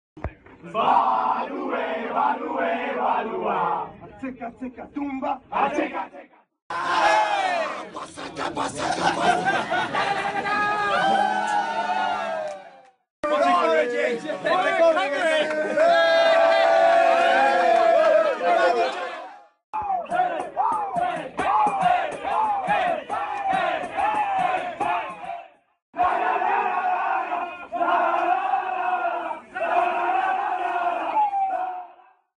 The celebration of the Timbers in the locker room 2015
Portland players had fun in the locker room after winning the MLS Cup.

Los jugadores de Portland se divirtieron mucho en el vestuario tras conquistar la MLS Cup.